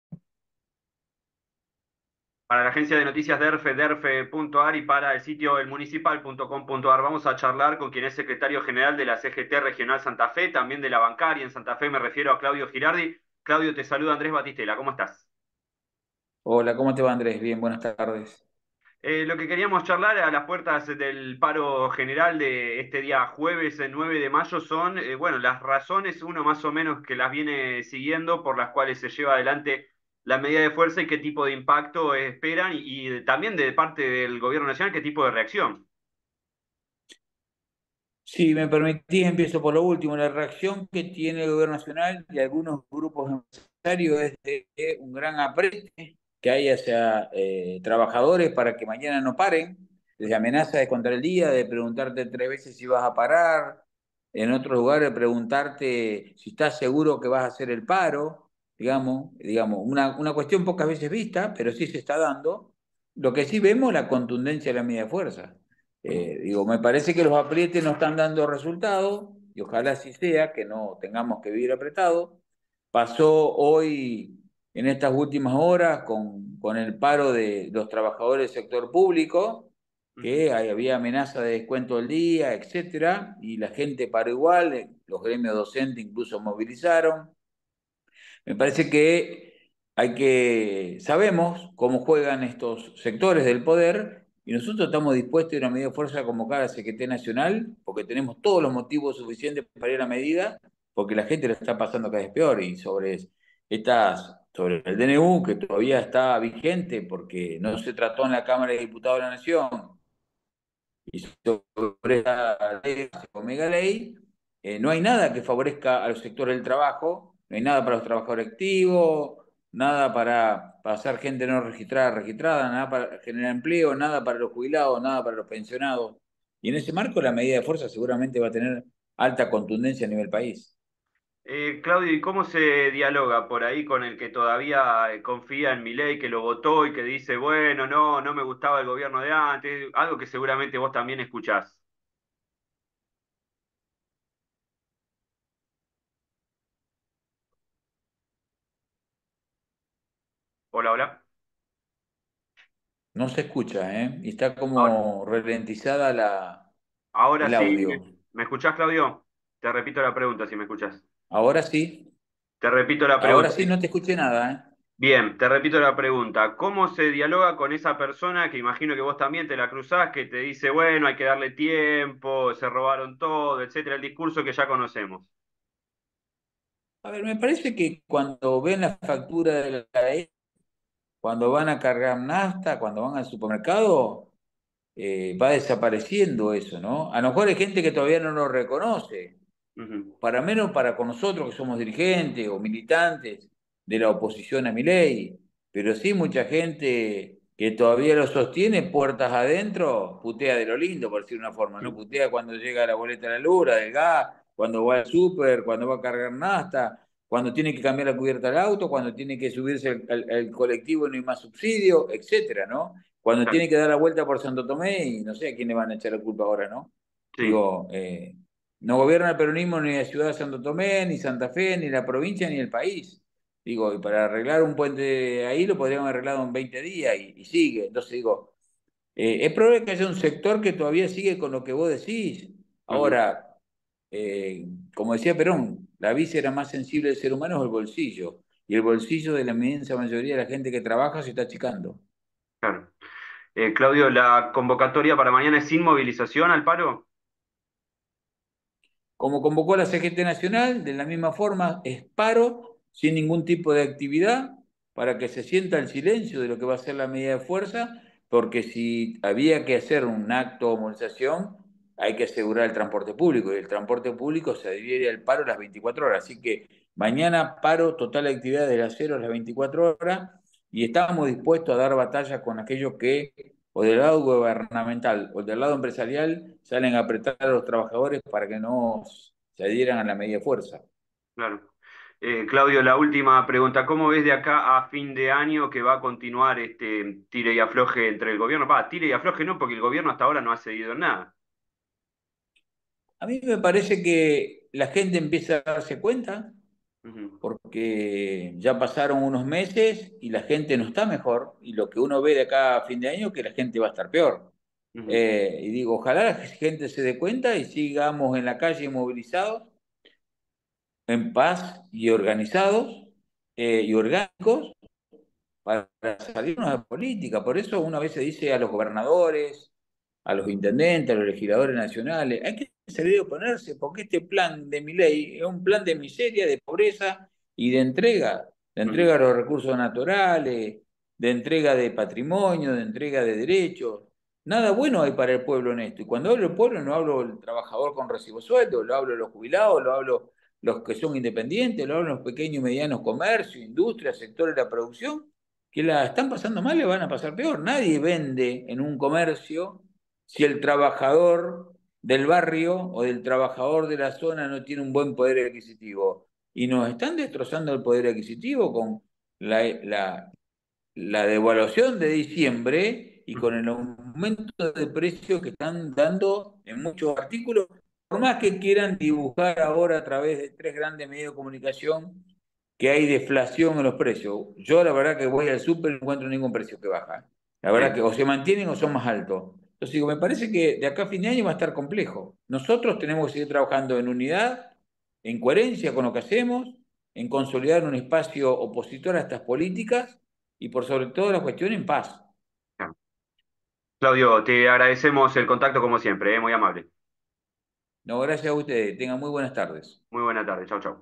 Claudio Girardi, sec. gral. CGT Santa Fe: "El Gobierno amenaza a los trabajadores que van al paro"
08/05/2024